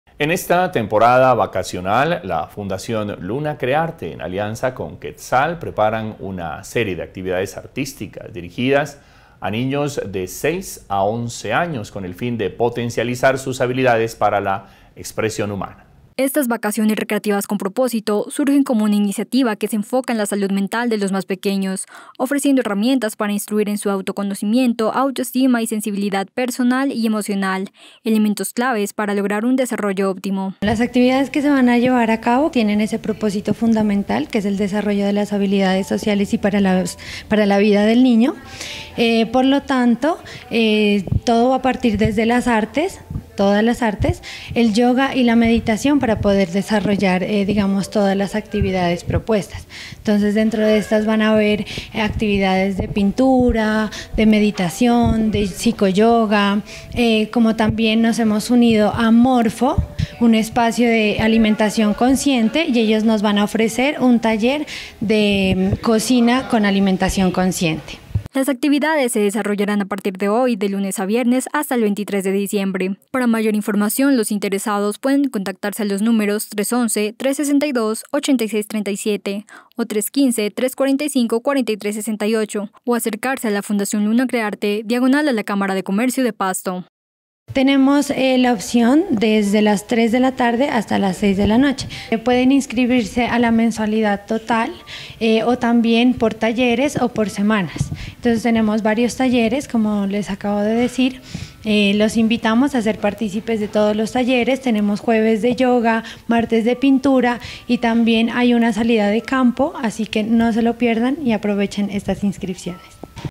🔴ACTUALIDAD INFORMATIVA| Temporada vacacional la fundación Luna crearte en alianza con Quetzal.
🔴ACTUALIDAD INFORMATIVA| En esta temporada vacacional la fundación Luna crearte en alianza con Quetzal preparan una serie de actividades dirigidas a niños de 6 a 11 años, con el fin de potencializar sus habilidades para la expresión humana.